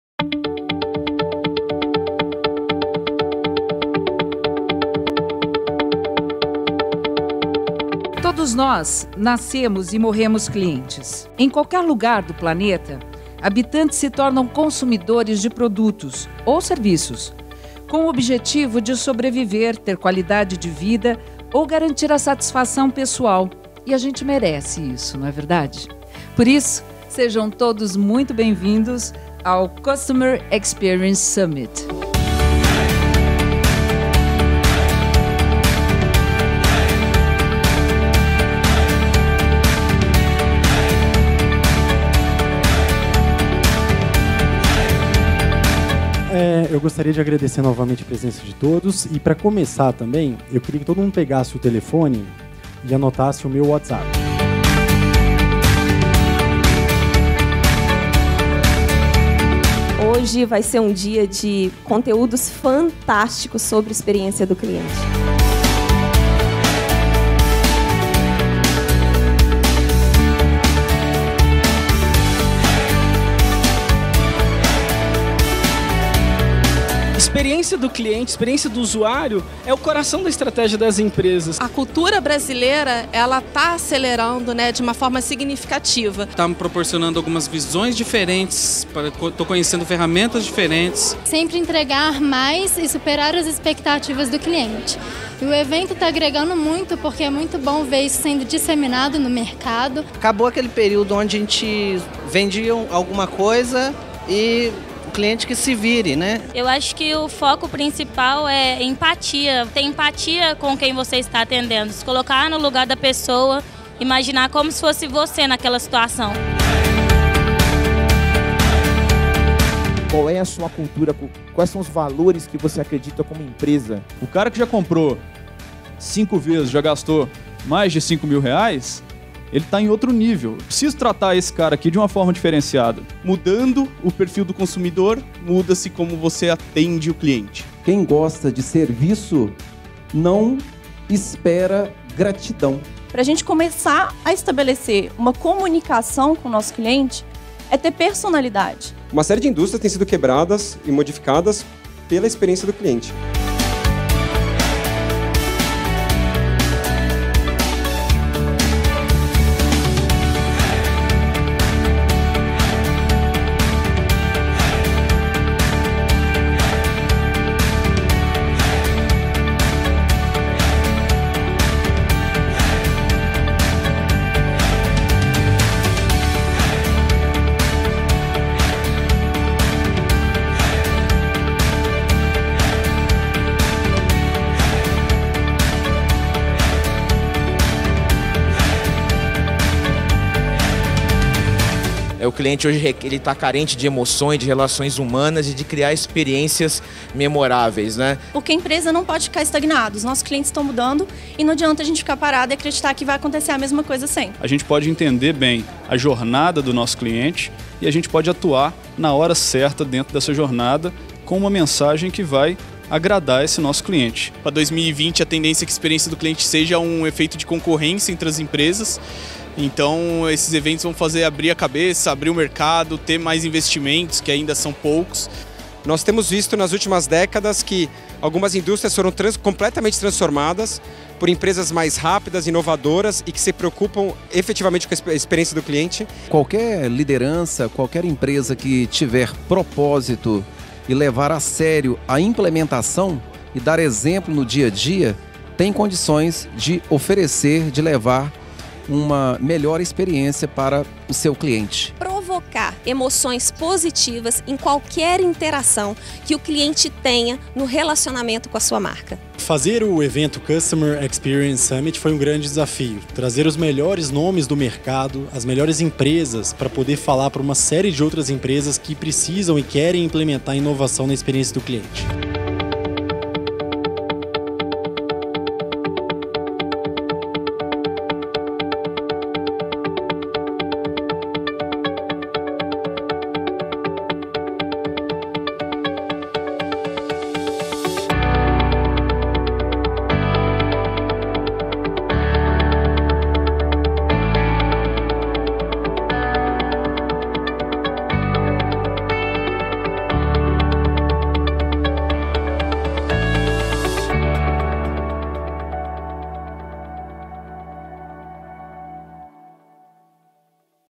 Customer Experience Summit BH 2016
Customer Experience Summit Belo Horizonte | 6 de Dezembro de 2016 | Instituto de Arte Contemporânea Inhotim

O CX Summit, maior evento sobre experiência do cliente do Brasil, é realizado pela Tracksale.

Conheça a Tracksale, ferramenta para gestão da satisfação de clientes em tempo real: